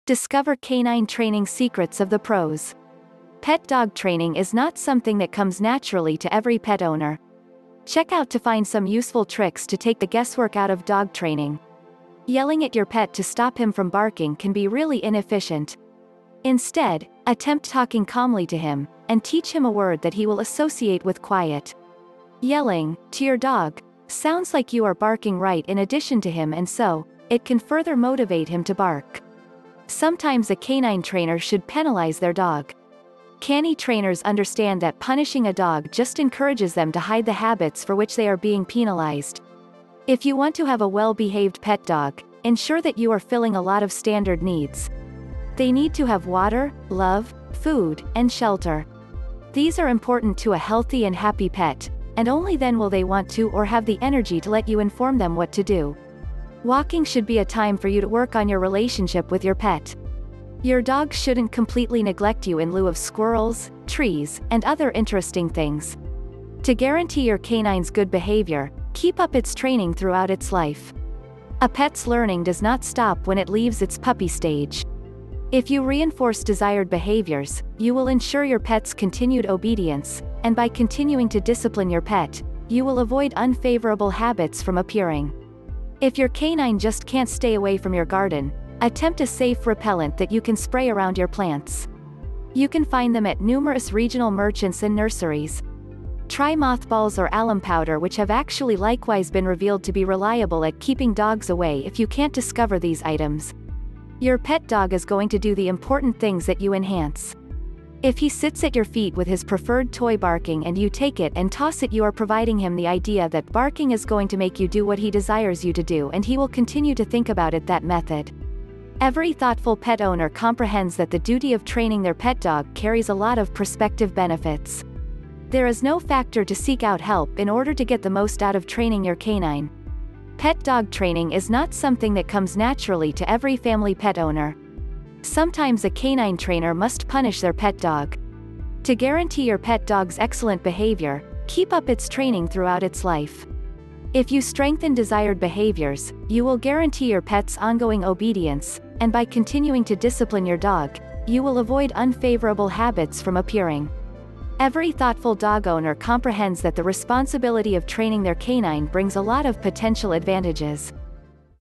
Discover Canine Training Secrets Of The Pros
Pet dog training is not something that comes naturally to every pet owner. Check out to find some useful tricks to take the guesswork out of dog training.
Yelling at your pet to stop him from barking can be really inefficient. Instead, attempt talking calmly to him, and teach him a word that he will associate with quiet. Yelling, to your dog, sounds like you are barking right in addition to him and so, it can further motivate him to bark.
Sometimes a canine trainer should penalize their dog. Canny trainers understand that punishing a dog just encourages them to hide the habits for which they are being penalized.
If you want to have a well-behaved pet dog, ensure that you are filling a lot of standard needs. They need to have water, love, food, and shelter. These are important to a healthy and happy pet, and only then will they want to or have the energy to let you inform them what to do.
Walking should be a time for you to work on your relationship with your pet. Your dog shouldn't completely neglect you in lieu of squirrels, trees, and other interesting things.